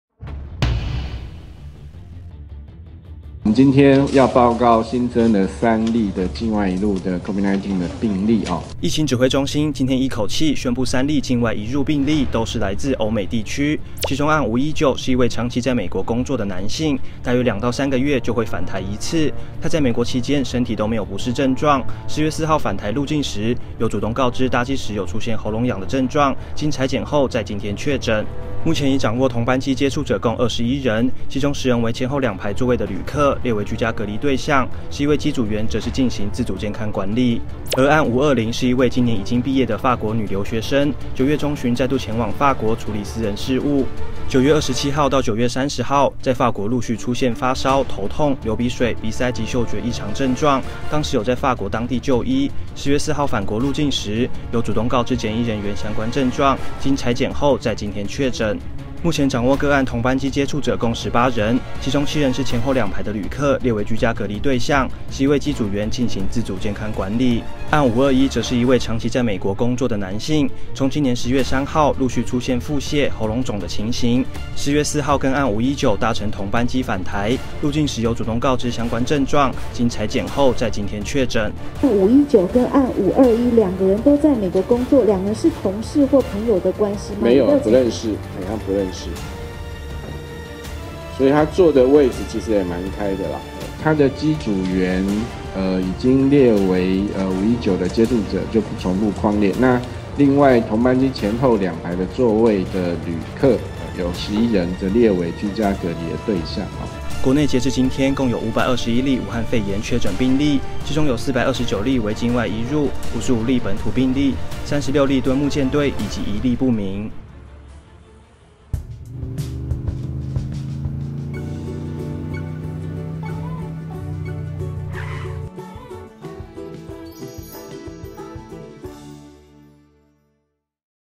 武肺疫報｜增3例境外移入 1女法國返台、2男自美搭同班機回台確診 | 蘋果新聞網
中央流行疫情指揮中心10/6宣布新增3例境外移入個案，由發言人莊人祥在14時30分臨時記者會說明。案519、521為30多歲男性自美國返台，案520為20多歲女性自法國返台，3例均為本國籍。國內累計521例確診。